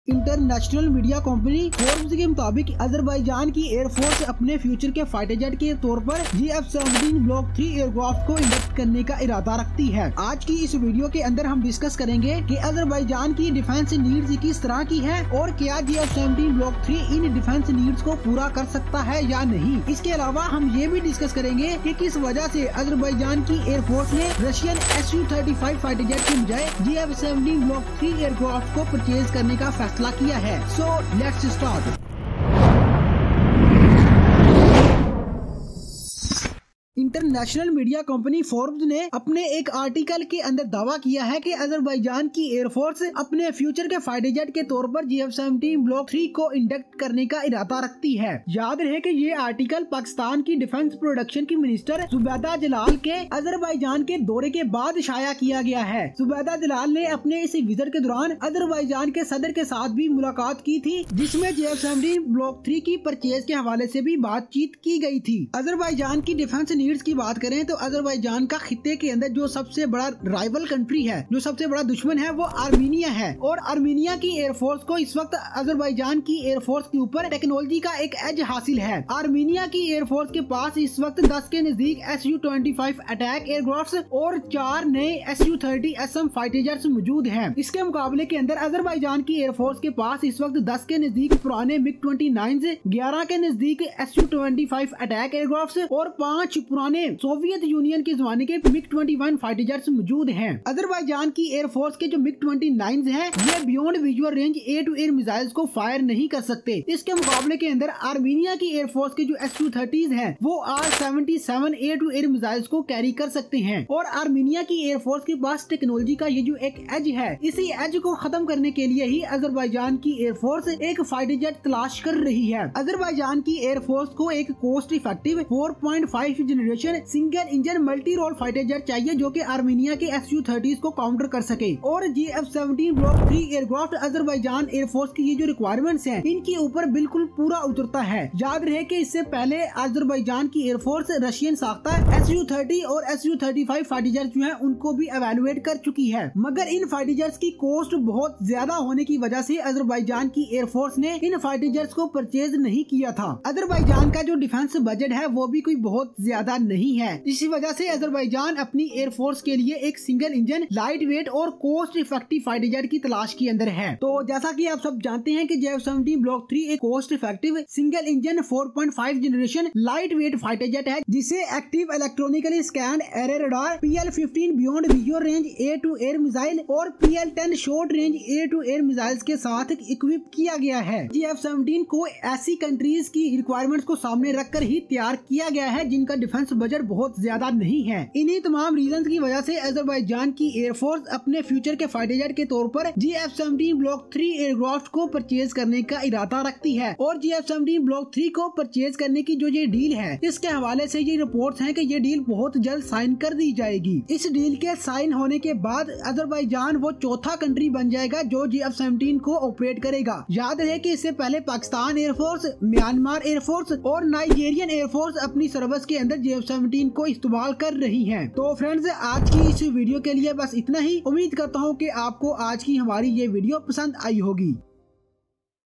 12 JF 17 Block 3 for Azerbaijan || Why Azerbaijan choose JF 17 over SU 35? Armed Version
JF 17 Block 3 for Azerbaijan || Why Azerbaijan choose JF 17 over SU 35? Armed Version

In this video we have discussed about Azerbaijan Air Force needs and why Azerbaijani decided to purcase JF 17 Block 3 over Russian SU 35.